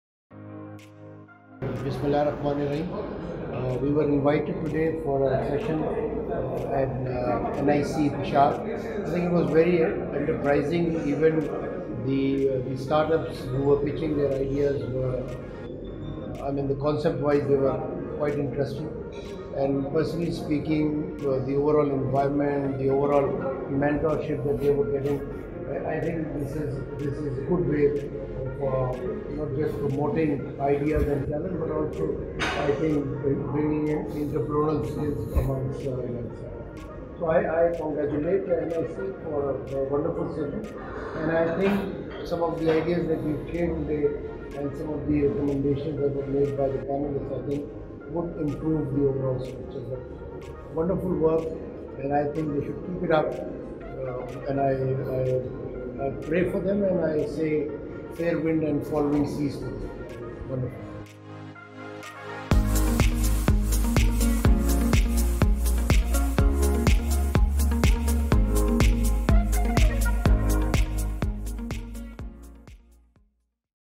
Hassan Daud Butt
Hassan Daud Butt, CEO of KP-BOIT, was invited over at NIC Peshawar among the judging panel for the selection of 6th Cohort Startups. He was very much impressed by the range and scale of ideas being presented. Here is what he said after the session.